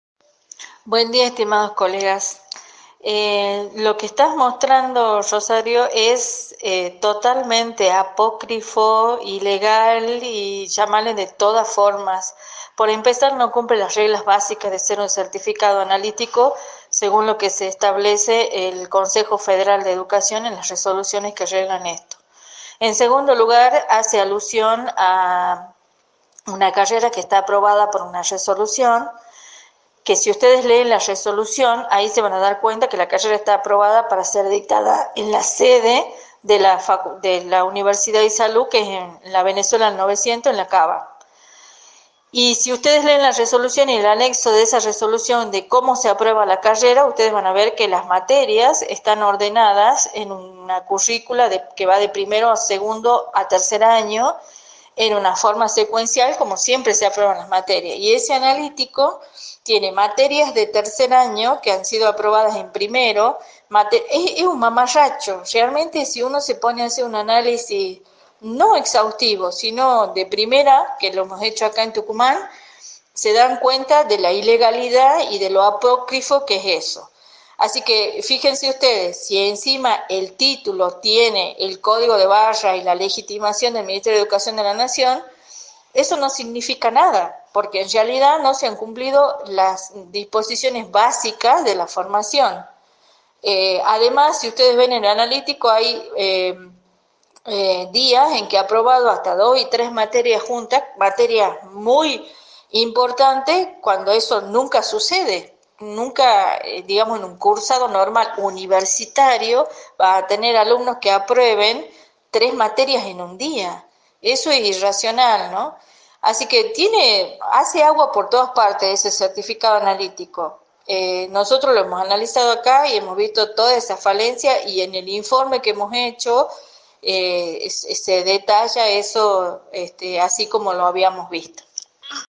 Achin De la Fuente Títulos Truchos
Días atrás, el titular de ATSA (gremio de enfermeros) hizo ir al Gobernador al Paseo Cultural para hacer la entrega de 'Títulos' a nuevos enfermeros. Desde el Colegio Profesional de Enfermería de La Rioja denunciaron que dichos títulos son 'apócrifos e ilegales', por no cumplir con las reglamentación básica que exige el Ministerio de Educación. ESCUCHÁ EL AUDIO y LEÉ EL COMUNICADO OFICIAL!!!